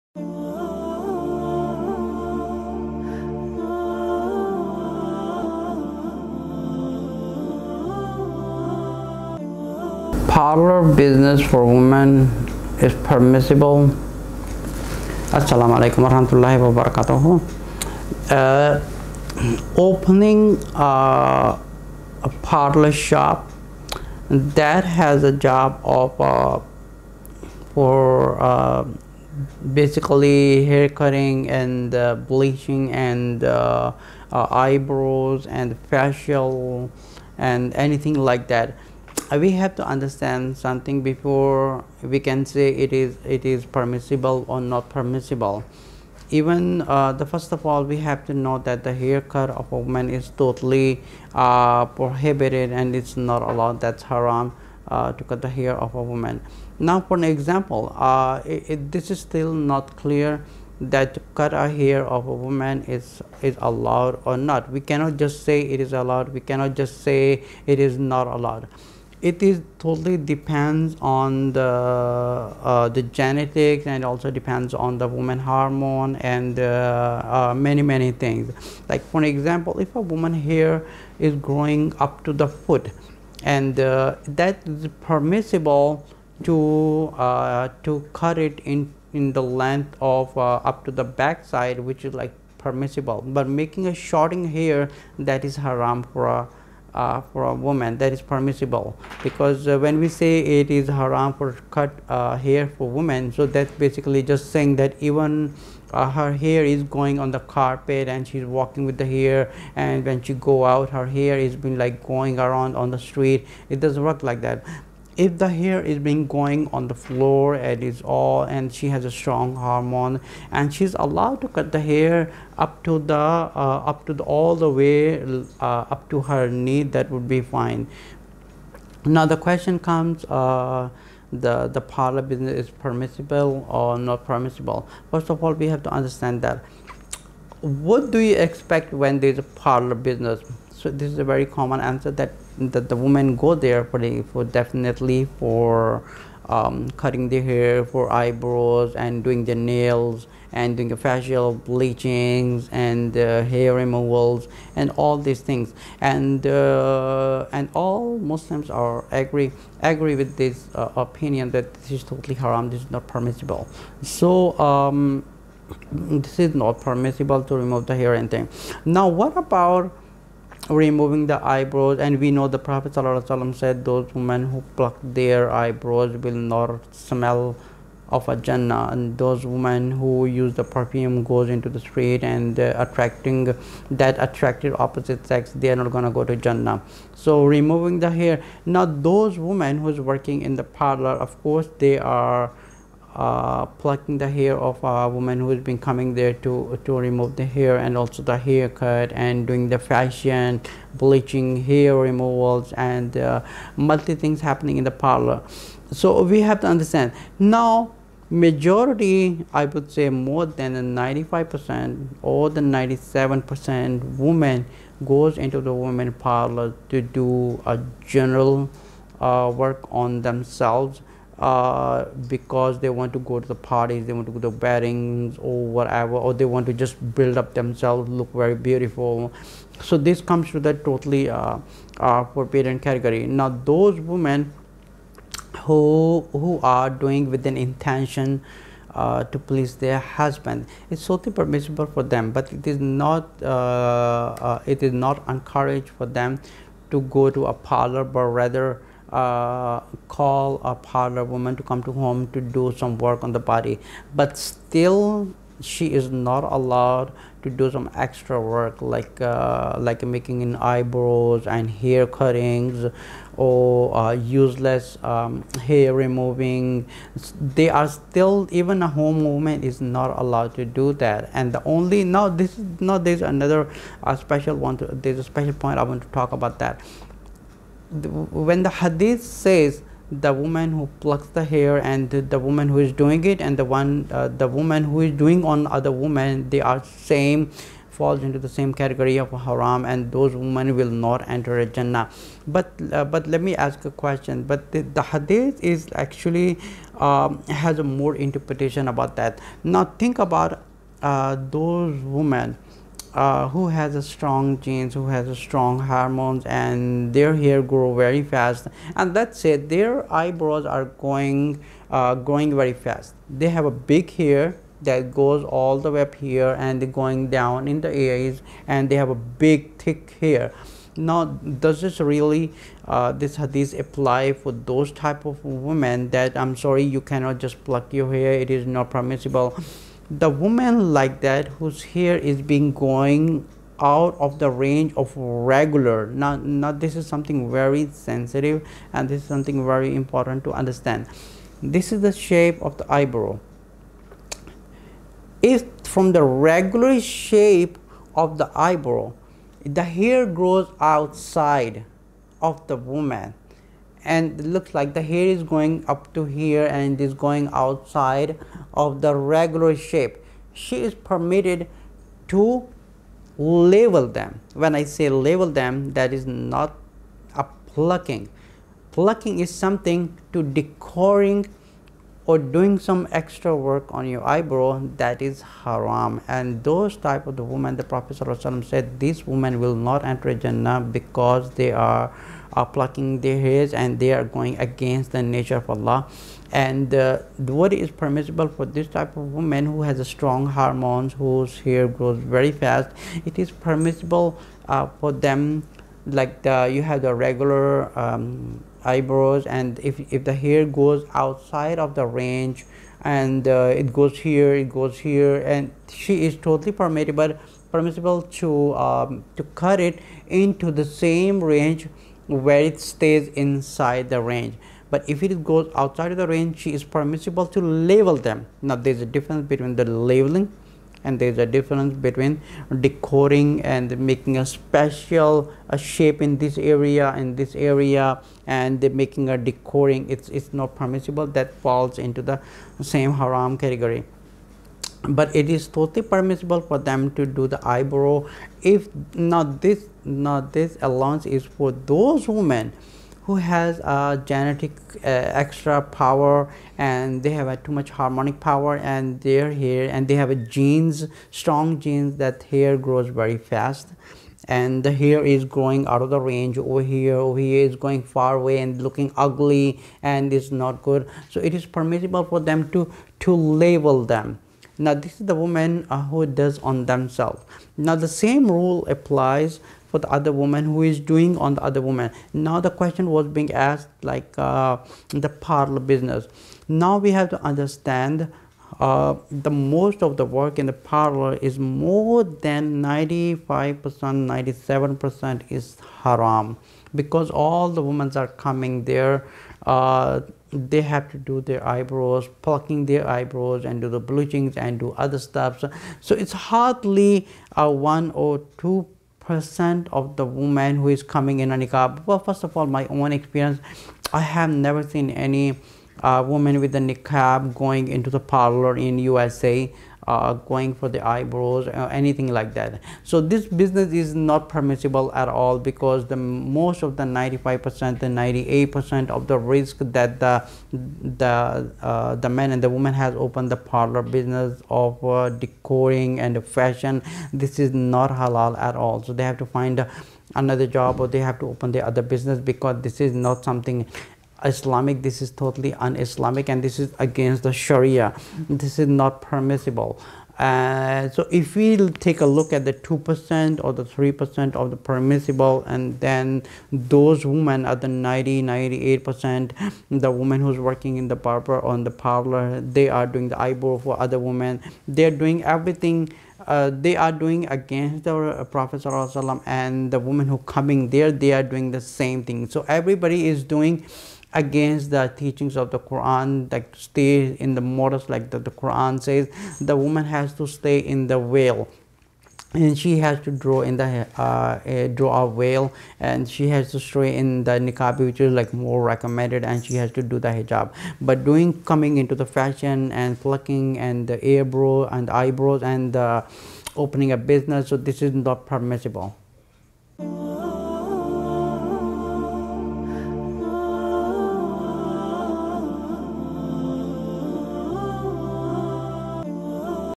Beauty Salon Parlor Business Hair Cut Plucking Eyebrow Rizq Makeup Haram || Mufti Ammaar Saeed
Mufti Ammaar Saeed
IRFNY